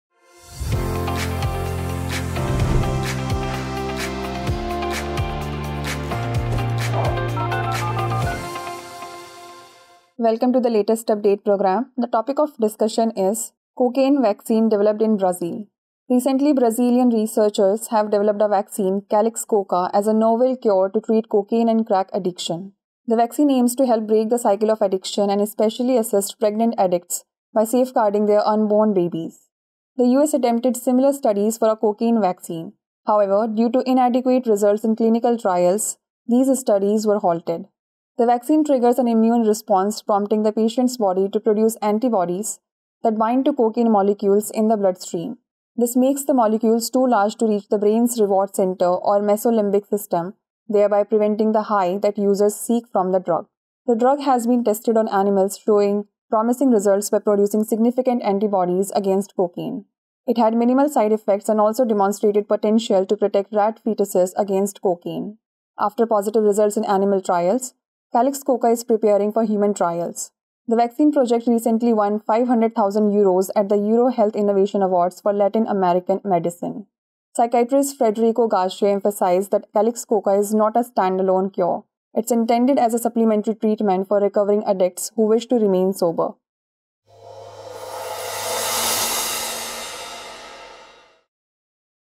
Cocaine Vaccine Developed in Brazil | Latest update | Drishti IAS English
Brazilian scientists have developed "Calixcoca," a vaccine that prevents the "high" from cocaine by triggering an immune response that binds cocaine molecules, stopping them from reaching the brain. After promising results in animal trials, the vaccine is set to undergo human testing and recently won a top prize at the Euro Health Innovation Awards.